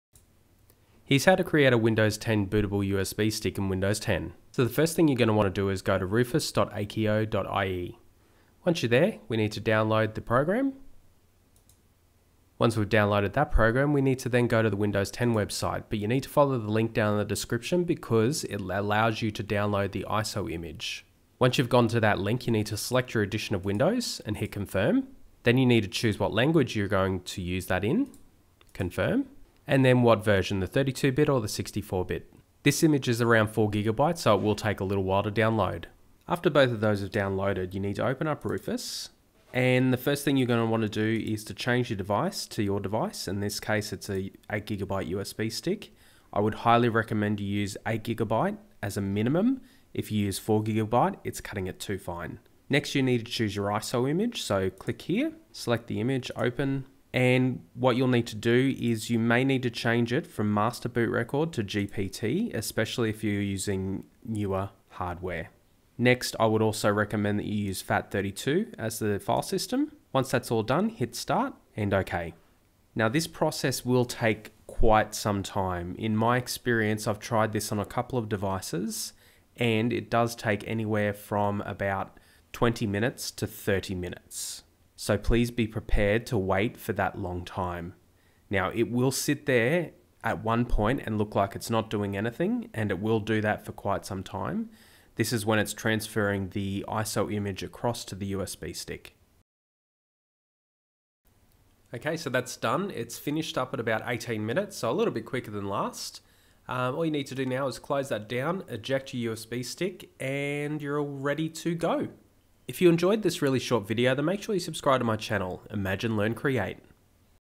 How To Make A Bootable Windows 10 Anniversary Update USB Installer
Do you want to know how to make a bootable Windows 10 Anniversary Update USB Drive in under 2 minutes?

Making a USB Boot Drive for the Windows 10 Anniversary Update is really easy. So easy in fact that I didn't want to make a full video. Instead, I've made this into a 2 Minute Tuesday video.

Below is a couple of important links to make your USB Drive bootable for the Windows 10 Anniversary Update

Here is the Download Link for Windows 10 Anniversary Update ISO

EDITOR: Kim Groth